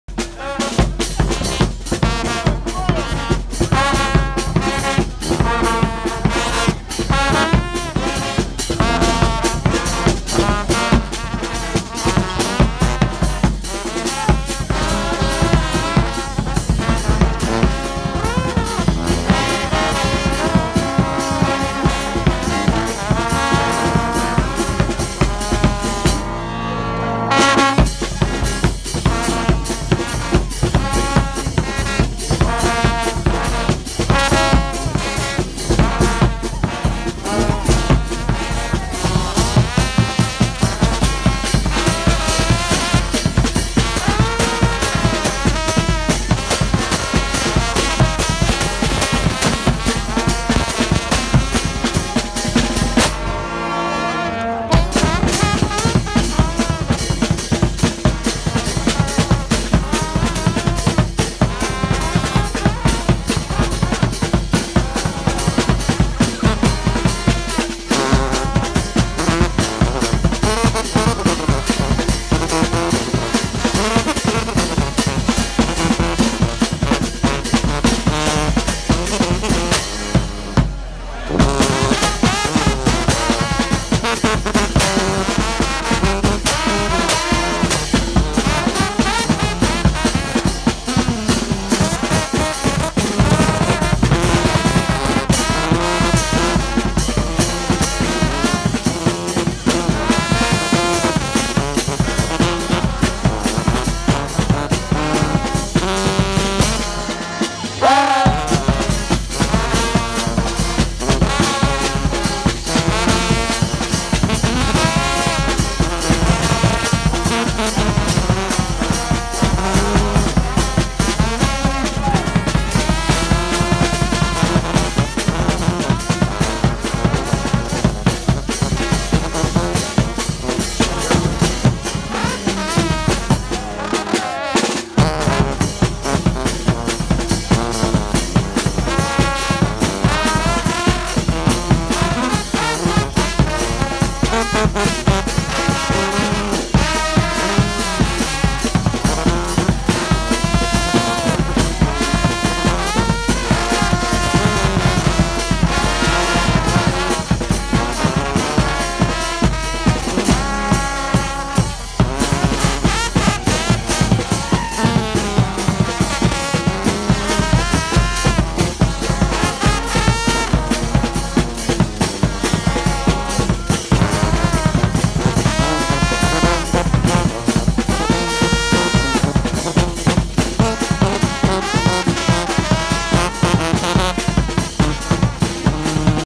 Mob with saints 2019
Chain gang - 2nd Street, peanut on bass horn & Thomas the bass drum